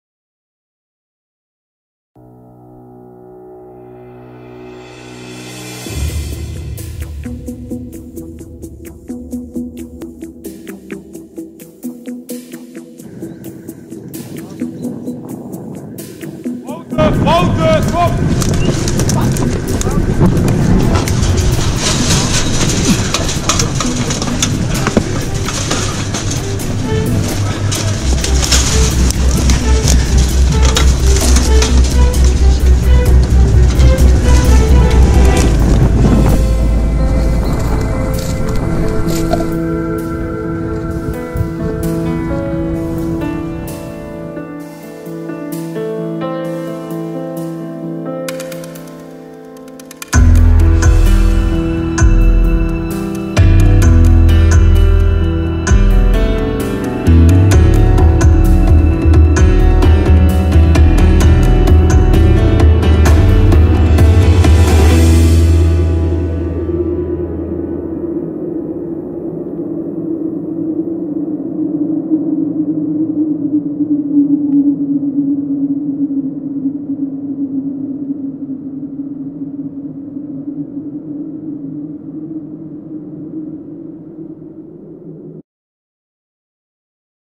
Oostvaardersplassen - Actievoerders - Stille tocht
Oostvaardersplassen - Actievoerders Staatsbosbeheer onder vuur!
17 maart 2018 Stille tocht.